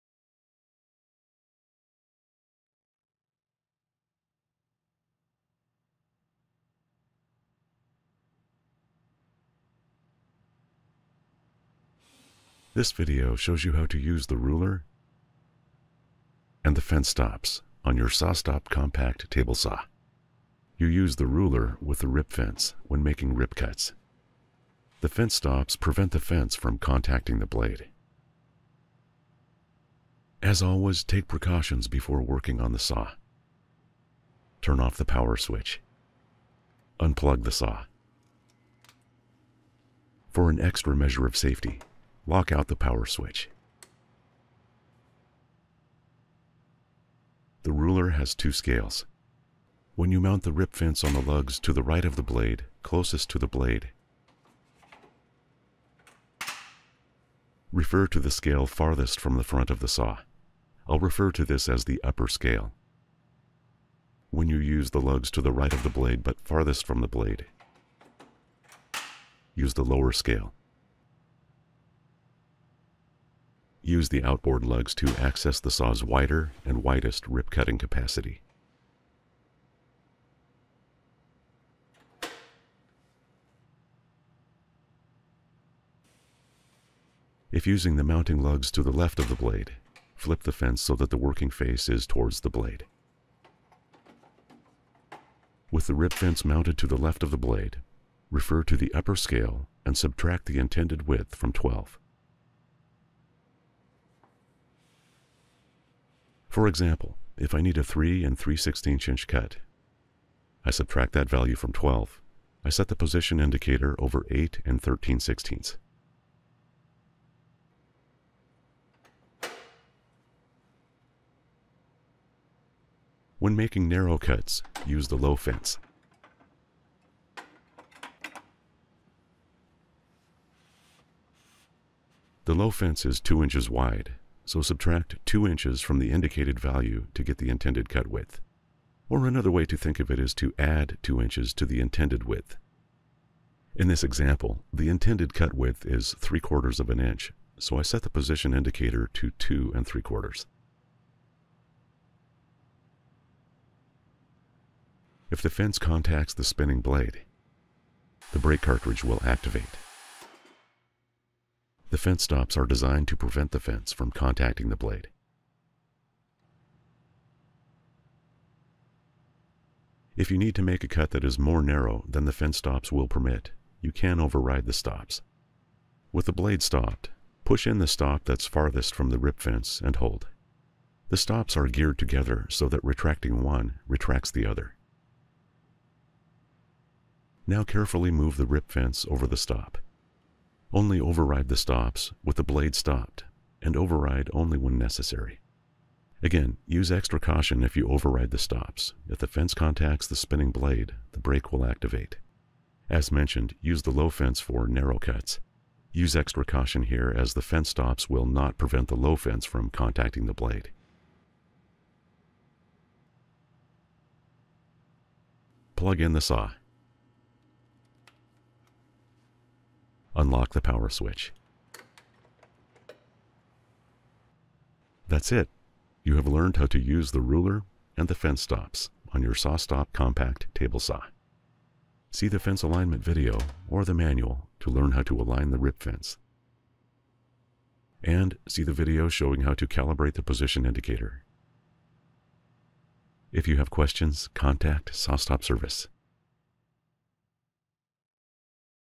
SawStop Service Tip: Using the Ruler and Fence Stops on the Compact Table Saw
This video details the SawStop recommended process for adjusting the fence stops on the Compact Table Saw (CTS-120A60 and CTS-230A50I).

For more information on maintaining and servicing your saw, please